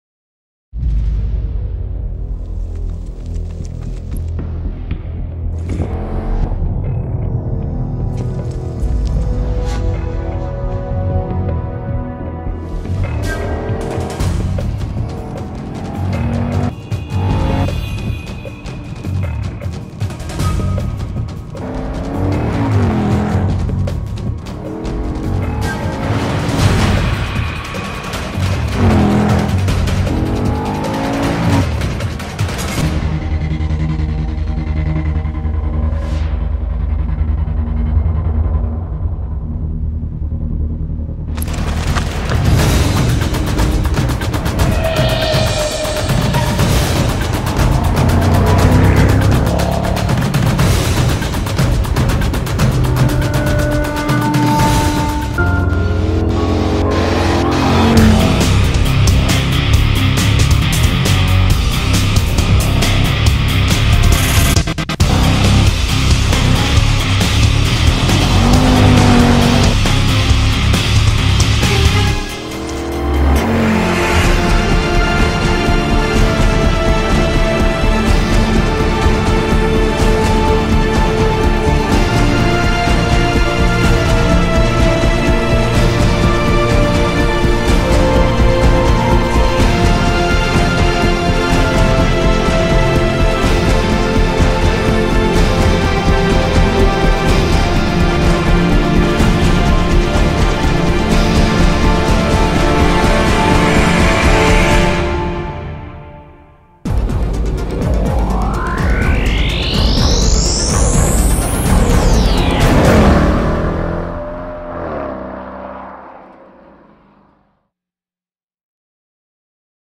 2012 Mercedes-Benz SLK 55 AMG
short performance clip with some good engine sounds at times.